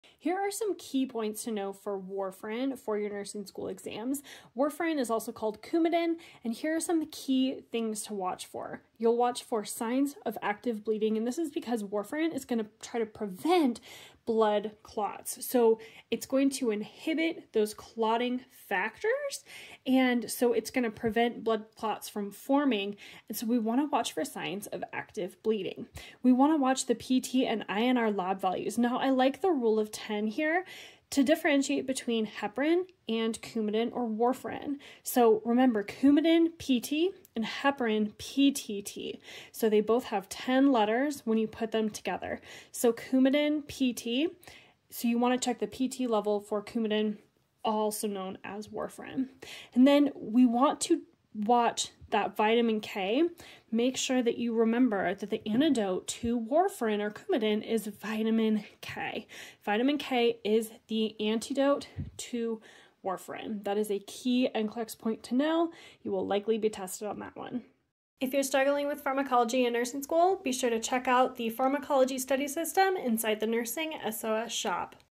Warfarin MUST-KNOW Key Points for Your Nursing School Exams and the NCLEX®
In this video, we're walking through the must-know warfarin (Coumadin) info that you'll need to know for your nursing exams and the NCLEX®!

🧠 MEMORY TRICK:
Use the "Rule of 10": Coumadin PT has exactly 10 letters, while heparin PTT also has 10 letters. This helps you never mix up which anticoagulant goes with which lab value!

💡 KEY CRITICAL THINKING POINT:
Always think about the mechanism of action when you're studying pharmacology! Warfarin inhibits clotting factors to prevent blood clots, so you'll want to watch for bleeding. Everything you monitor (PT/INR, bleeding signs, vitamin K) connects back to this anticoagulation effect.

🚨 NCLEX® KEY POINT:
Vitamin K is the antidote to warfarin/Coumadin! This is super likely to show up on your nursing exam or the NCLEX®! Also remember: warfarin = PT/INR monitoring.